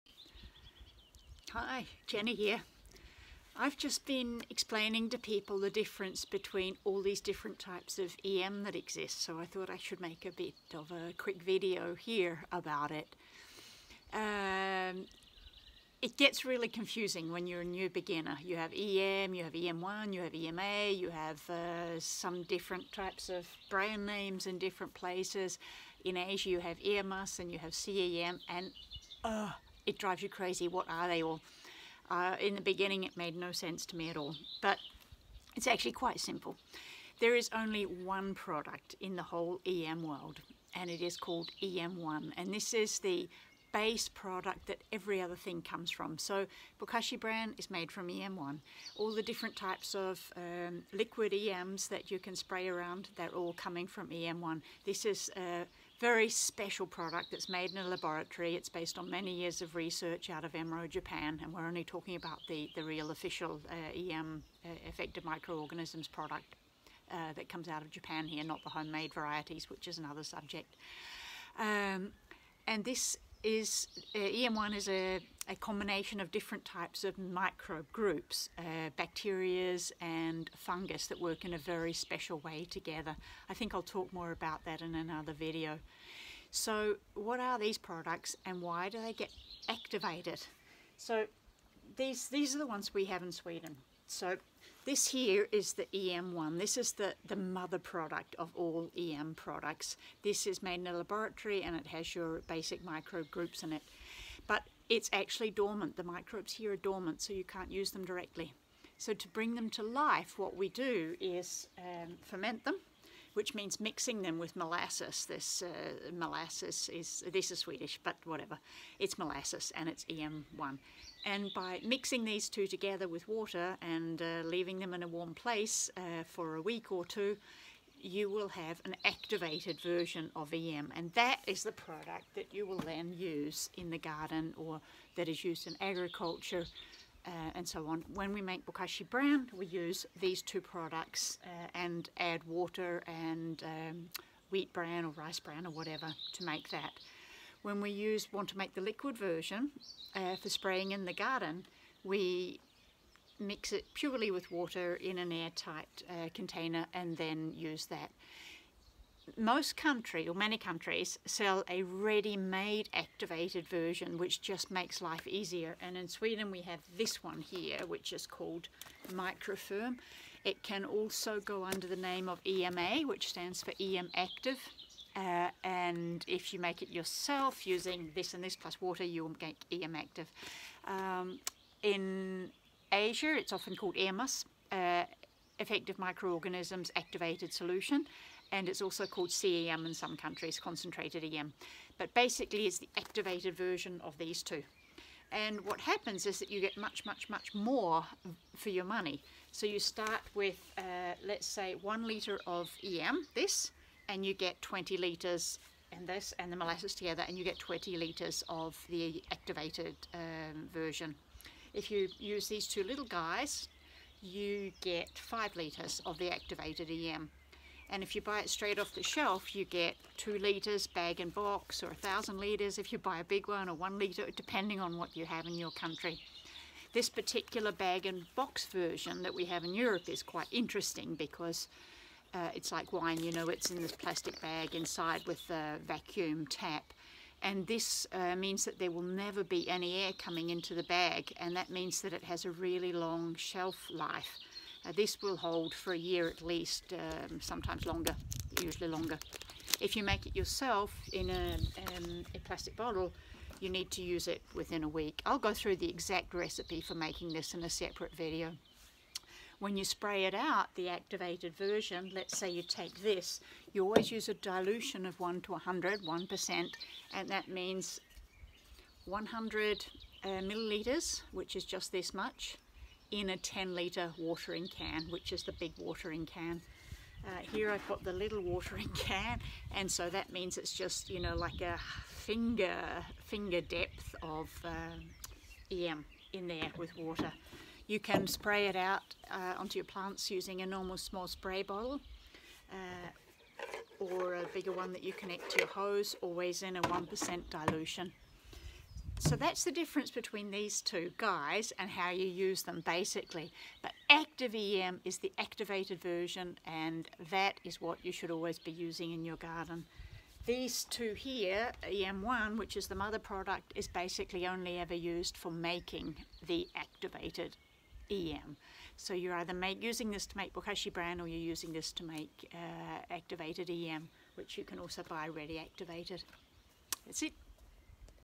What is activated EM and how do I use it?
EM goes under a lot of names and here I explain what they all are and how to use them.
I always use original EM-1 from EMRO Japan, as this has all the microbe groups and is what the research is based on.
It sounds complex but it’s not!
/Jenny